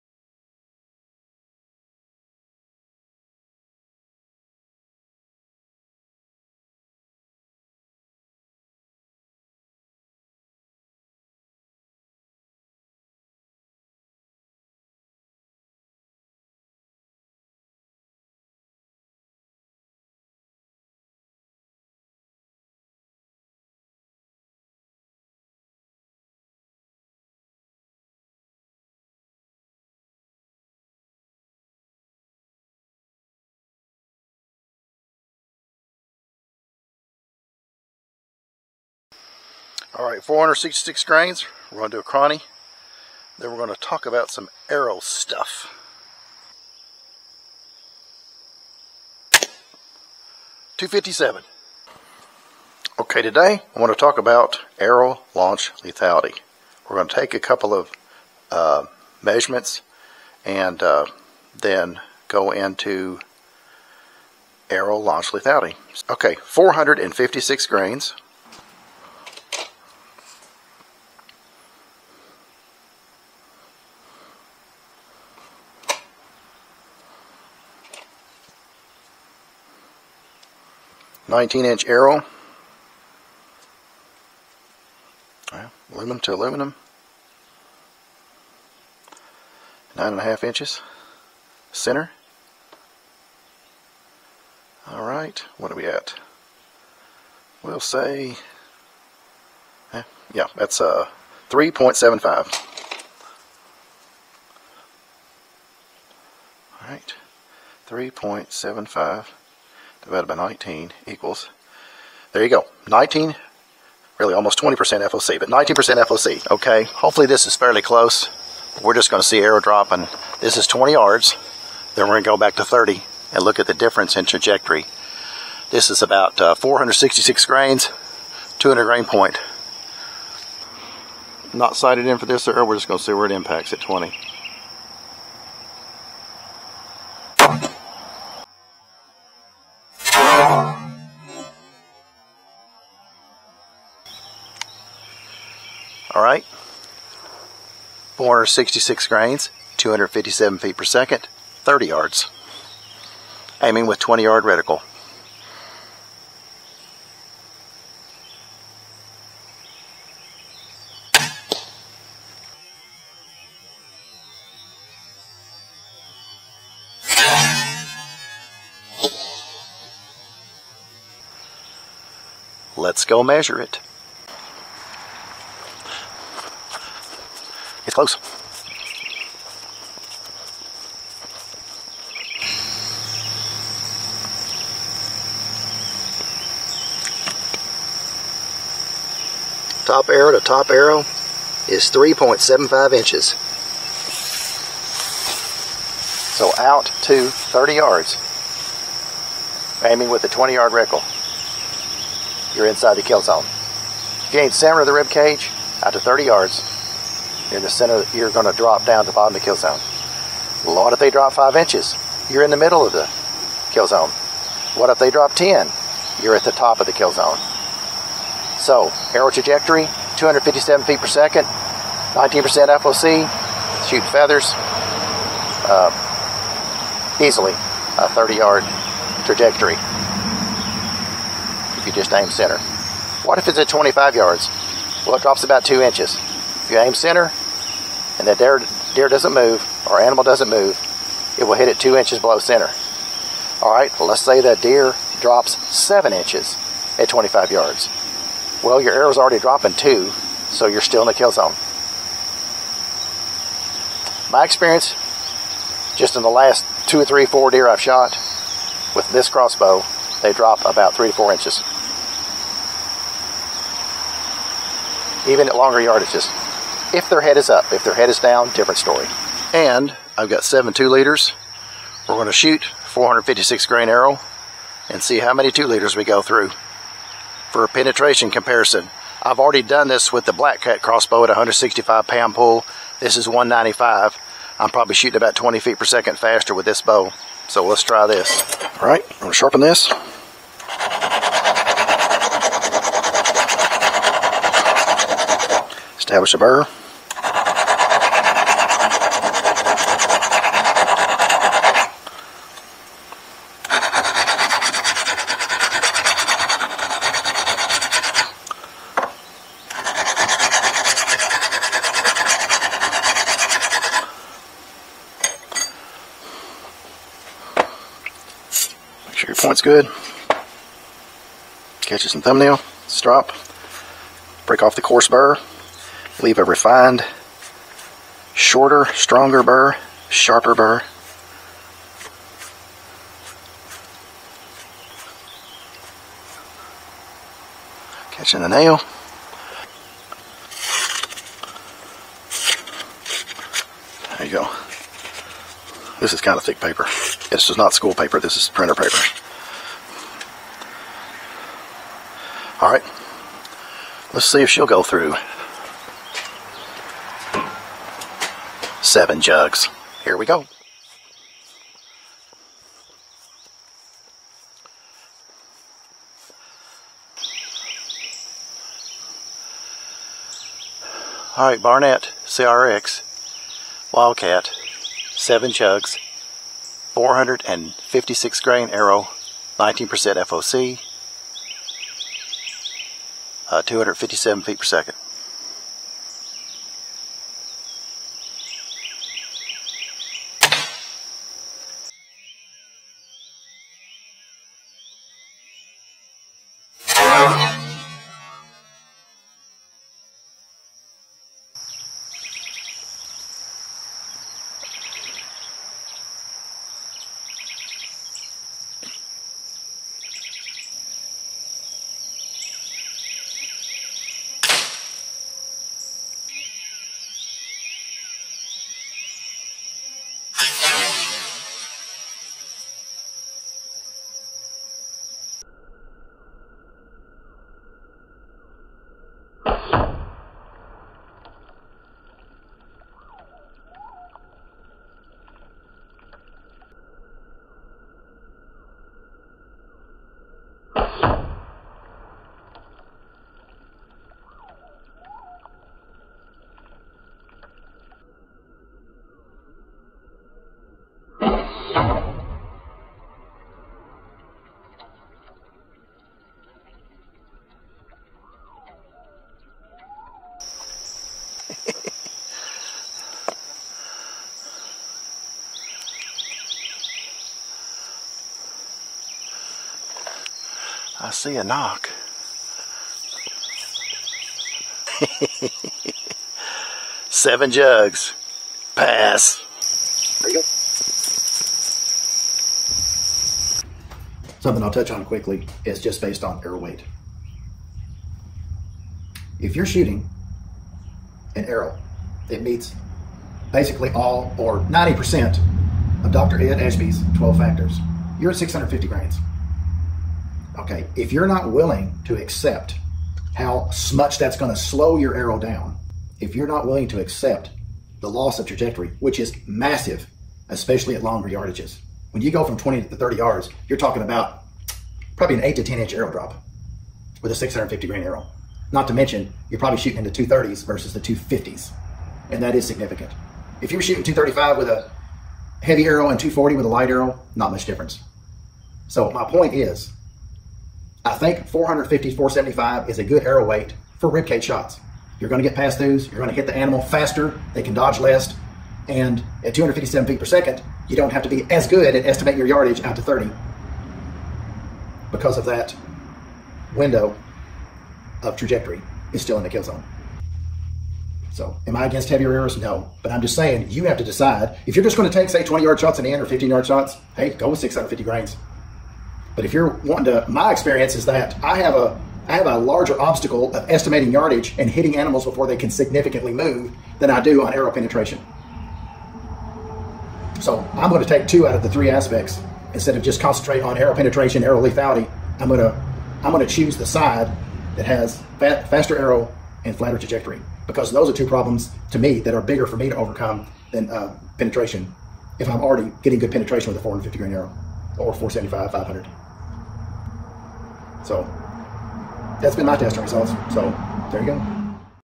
Barnett Wildcat CRX 466 grain Arrow Drop VIDEO FOUR
The fourth video in a series of FOUR VIDEOS. Arrow System Integrity, Arrow Velocity, and Arrow Trajectory all need to be considered in creating a more successful hunt outcome. The ARROW SYSTEM has to survive impact and have sufficient weight, proper design, and optimum flight to achieve adequate penetration. The ARROW VELOCITY has to be fast enough to reduce the amount of time the target animal has, to react and significantly change the impact location of the arrow. The ARROW TRAJECTORY (or arrow drop) has to be reduced to overcome the average person's inability to accurately judge shot distance at longer distances.